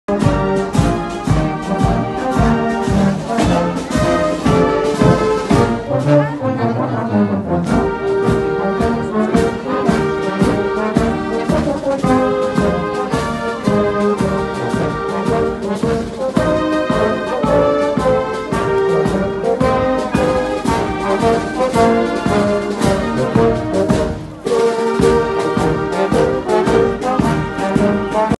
Dauwtrappen 2019 met OKK in Zevenbergschen Hoek
Traditiegetrouw startte Hemelvaartsdag ook dit jaar met het dauwtrappen. Onder begeleiding van Fanfare OKK liepen 64 deelnemers een rondje door de polder, waarbij ze onderweg genoten van een goed verzorgd ontbijt.